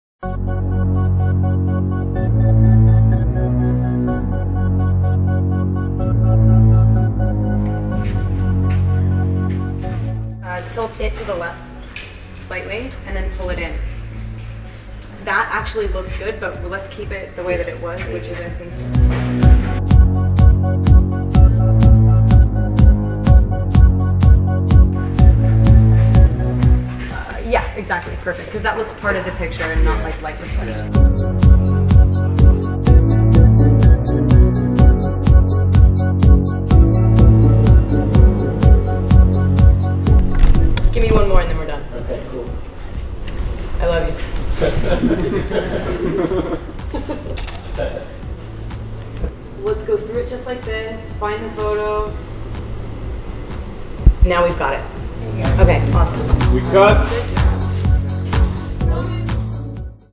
Kristen Stewart at Thunder Studios (COME SWIM)
Behind the scenes footage of Kristen Stewart's directorial debut short film, COME SWIM. Screened at Sundance 2017.